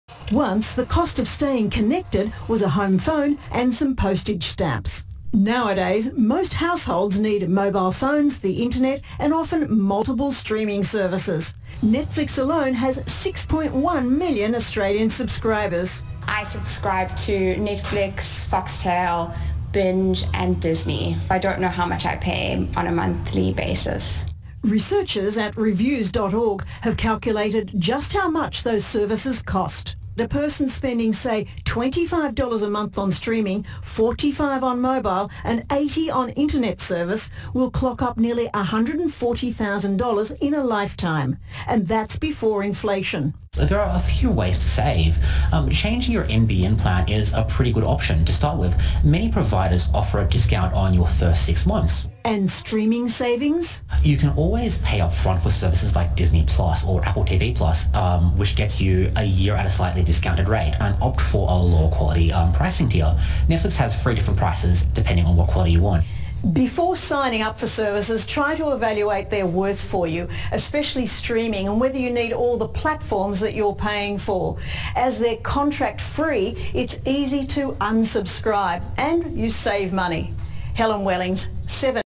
The cost of being connected - 7News
Alex Choros of WhistleOut talks to 7News about the lifetime costs of phone, internet and streaming services and how people could save money along the way.